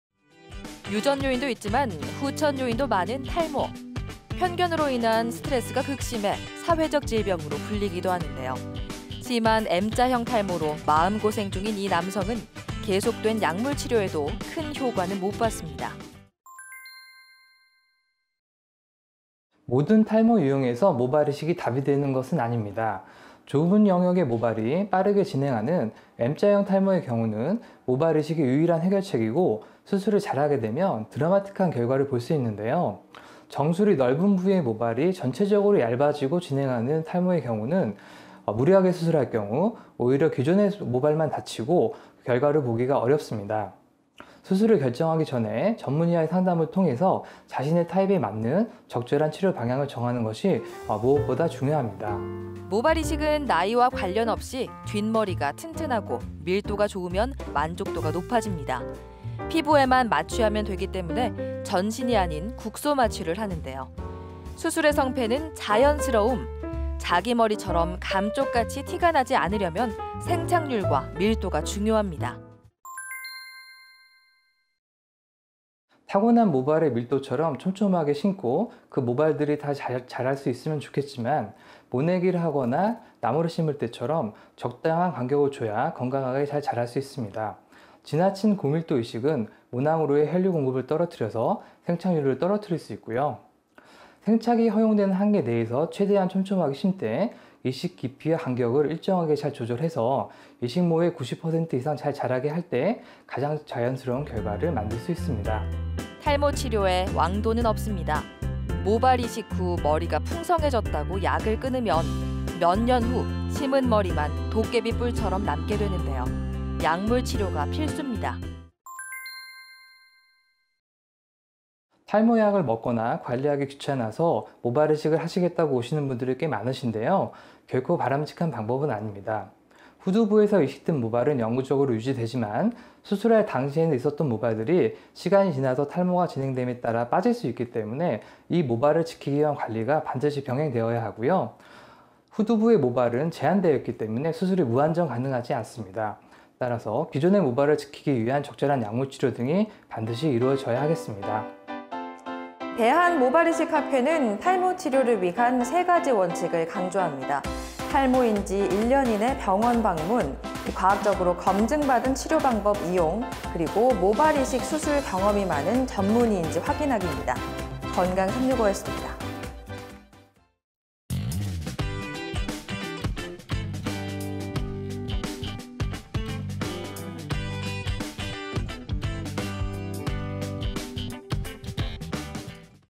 [건강365] 사회적 질병 탈모, 모발이식 언제 할까? 모발이식(김택훈 원장/ 051-806-5824)
(부산맥스웰피부과/ 김택훈 원장/ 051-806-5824)

매일 하루 3분 '건강365'